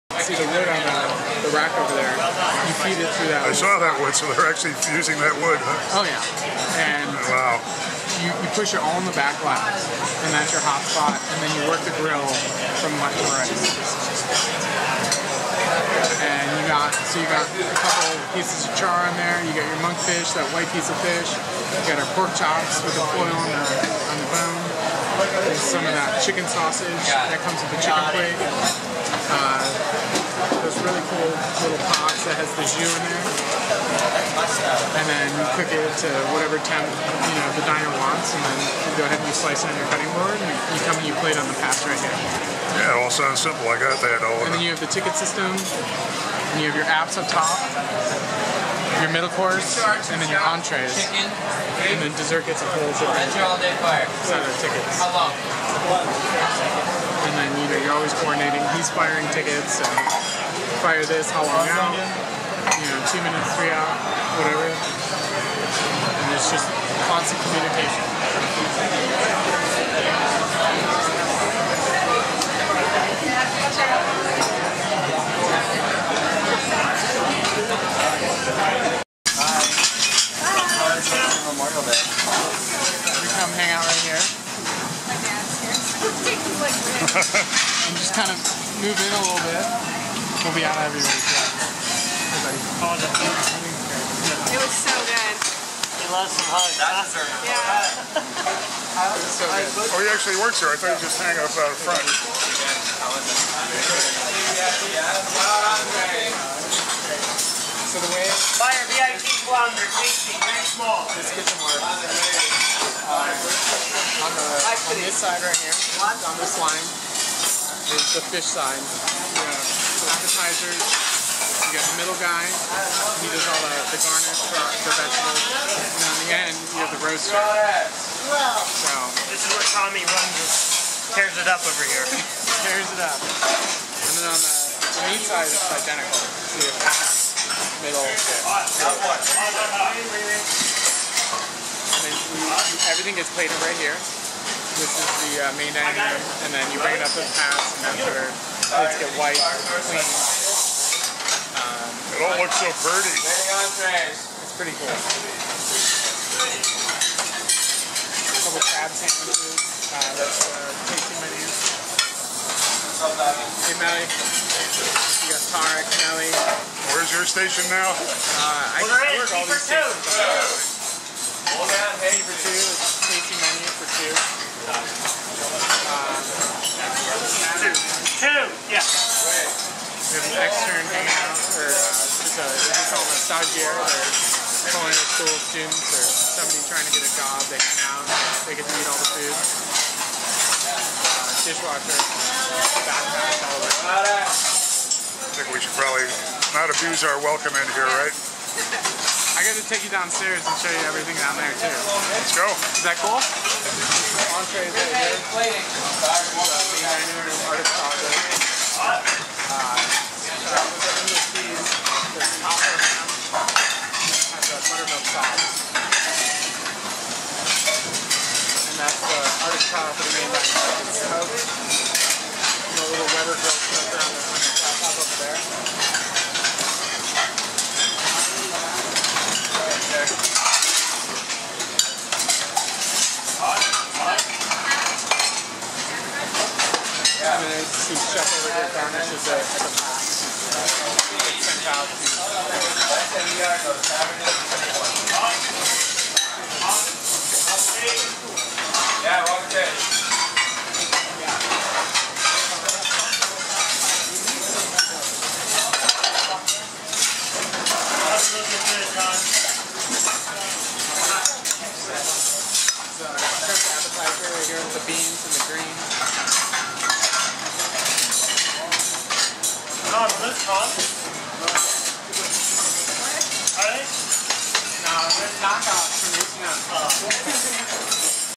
A cook's tour of Gramercy Tavern with Tom Rogers
Tom Rogers, my daughter Val's boyfriend, took us on a behind-the-scenes tour at Gramercy Tavern. The tour followed a spectacular meal, one of the best I've ever experienced. We also toured the basement, but I didn't shoot video there.

Thanks, Tom. And thanks to all the wonderful folks at Gramercy who stopped by to say hello.